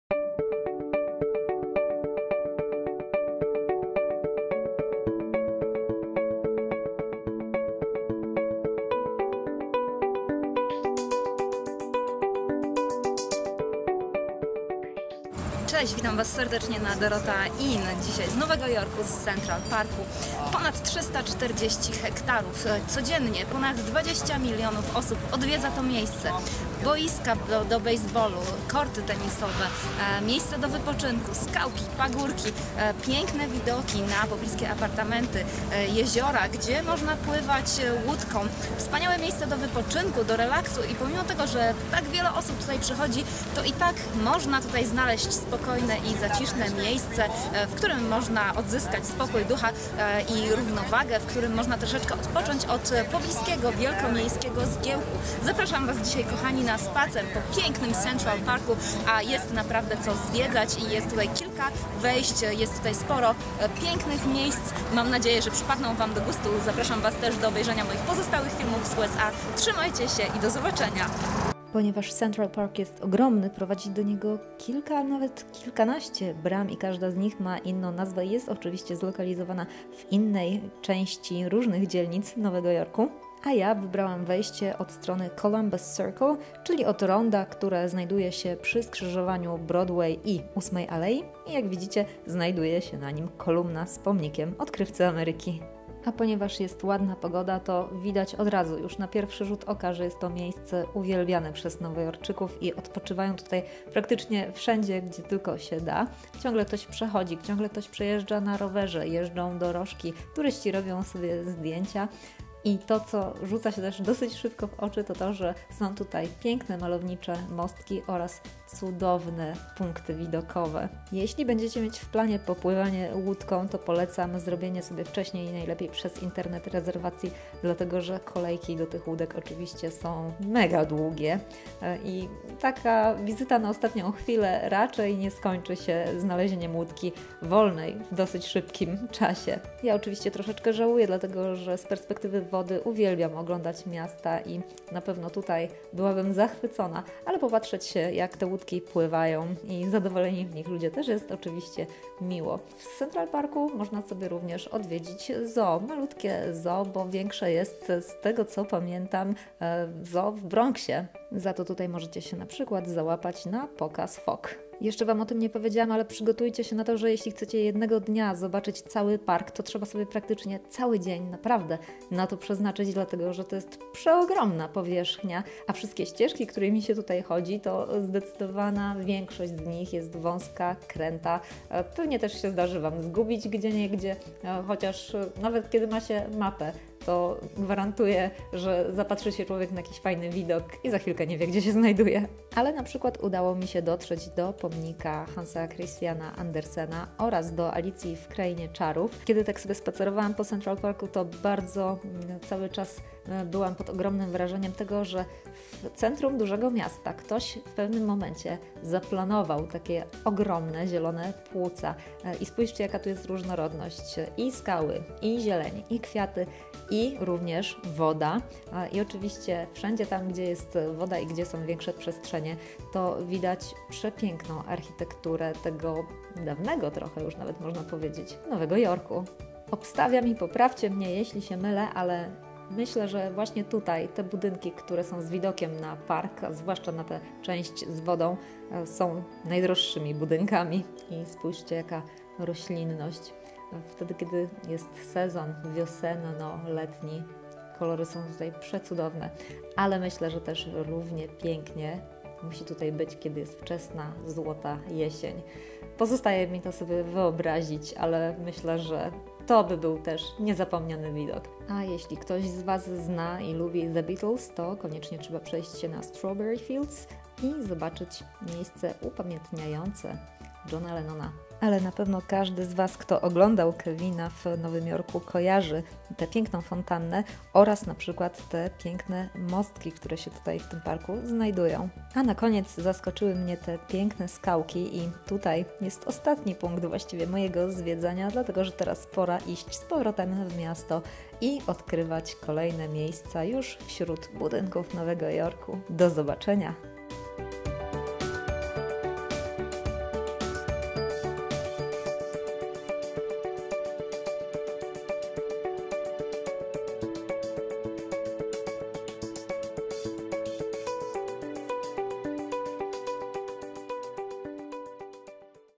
Central Park w Nowym Jorku 🇺🇸 relaks w centrum wielkiego miasta | Dorota Kamińska
Central Park w Nowym Jorku - vlog z długiego spaceru, a i tak nie byłam w stanie zobaczyć wszystkiego. Pokazuję Wam wiele ładnych miejsc i opowiadam o swoich wrażeniach.

Noclegi:

1. Tani nocleg w Nowym Jorku
2. Trochę lepsze warunki i nieco wyższa cena
3. AirBnB ze zniżką 100 zł